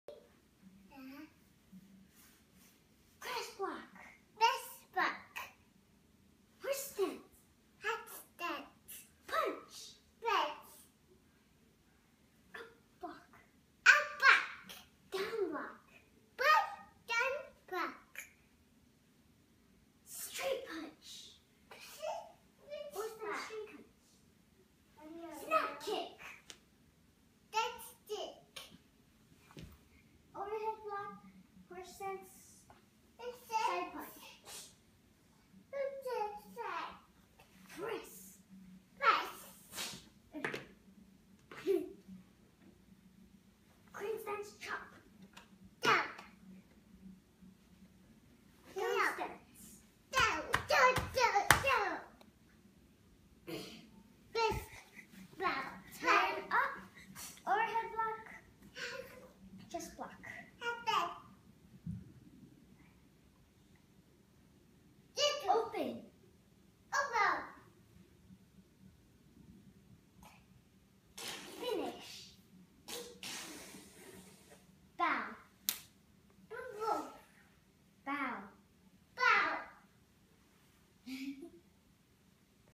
matthew teaching micah kung fu
big brother matthew (7 years old) teaching kung fu little brother micah (2 years old)